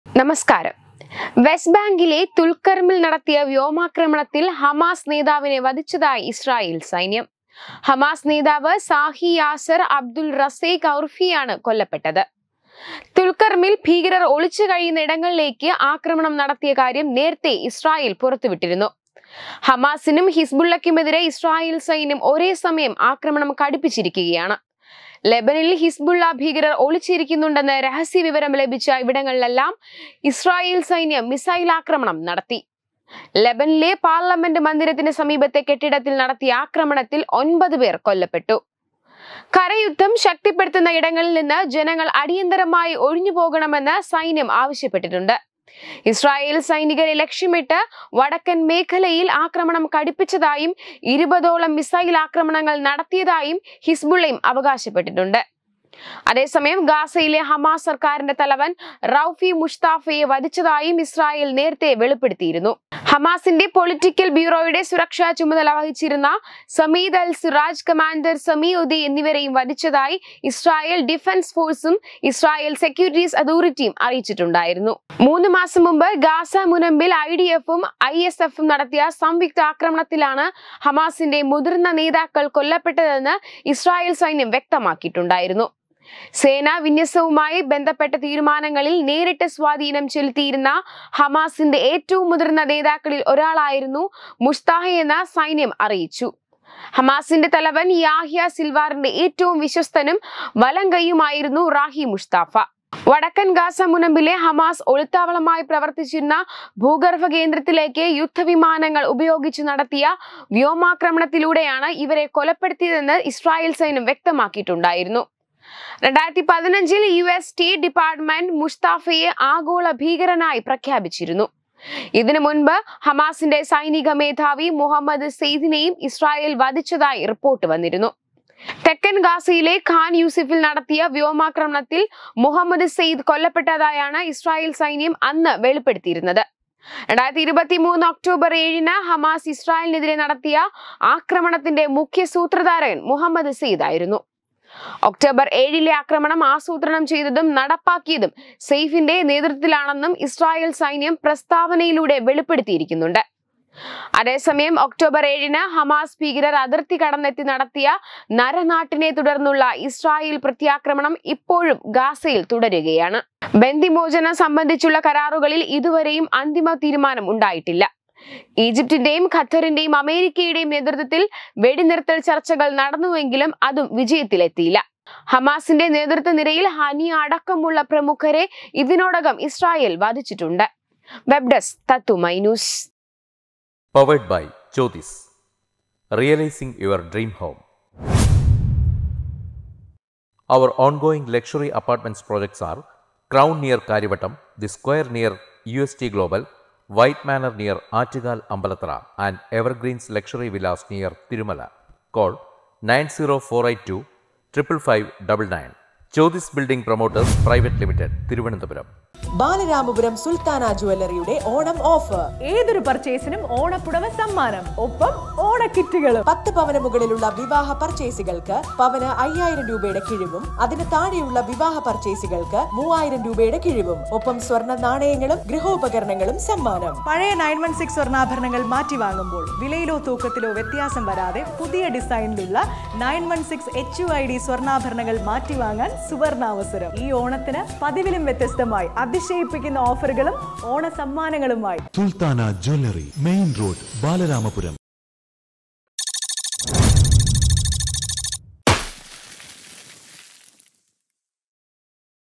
ഹ_മാ_സിനെതിരെ ഇസ്രയേലിന്റെ പോ_രാ_ട്ടം ശക്തം | ISRAEL
ഹ_മാ_സ് നേതാവ് സാ_ഹി യാസർ അ_ബ്ദു_ൽ റസേ_ഖ് ഔർഫിയെ  കൊ_ല_പ്പെ_ടു_ത്തി